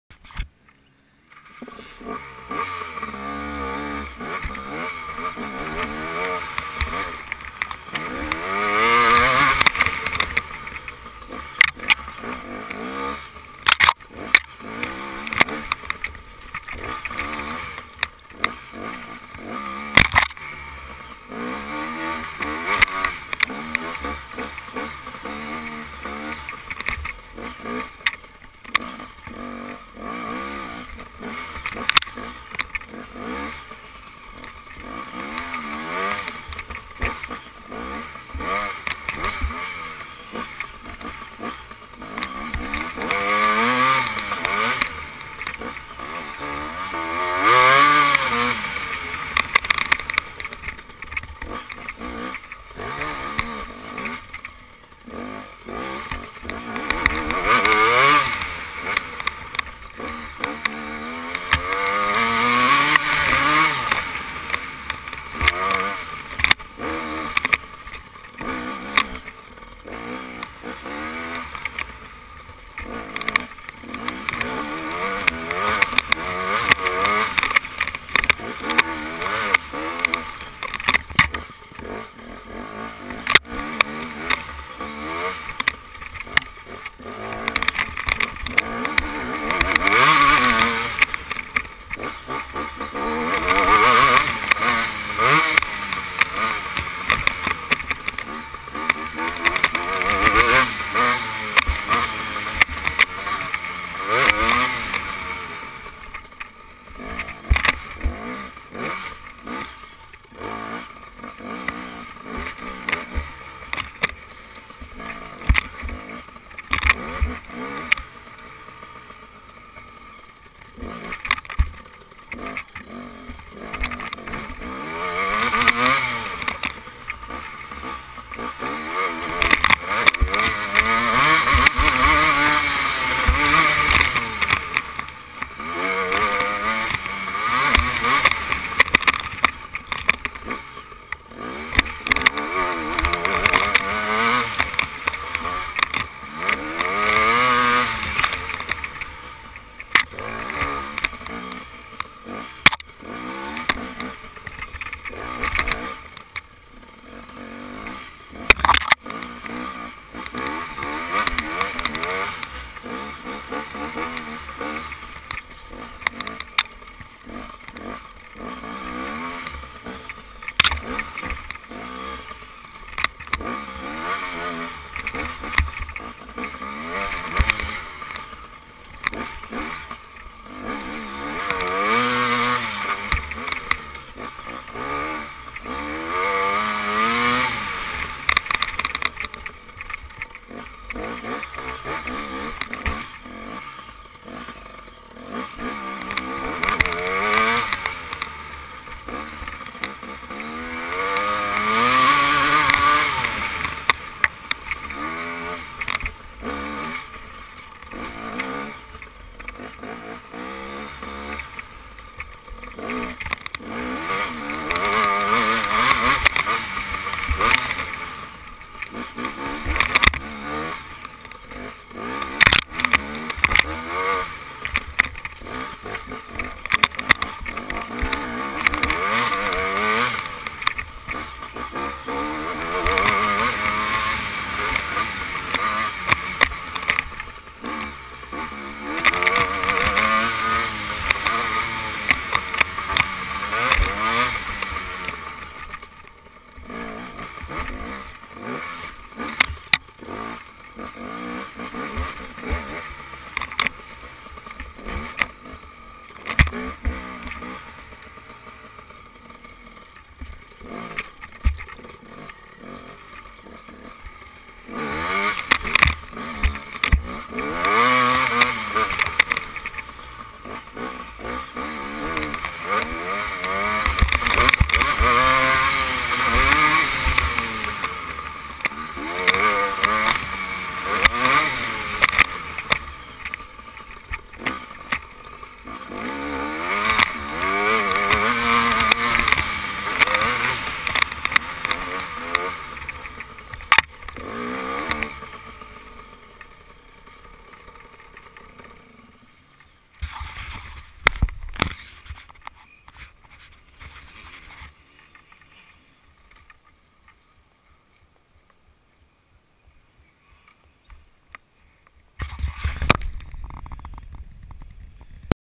McFall Endurocross/Harescramble/Motocross Compound
I just redid the entire track to have a little bit of everything. This is my first ride on it since I did all the work